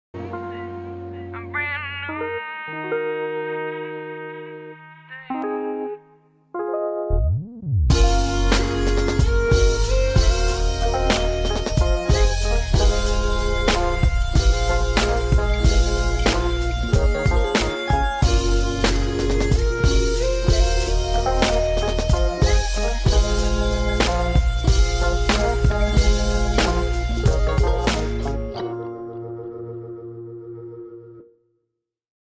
Manual Focus Test Cannon 60D (cannon ef 17-85mm)
Me trying to do a manual focus test with my Cannon ef 18-55mm lens.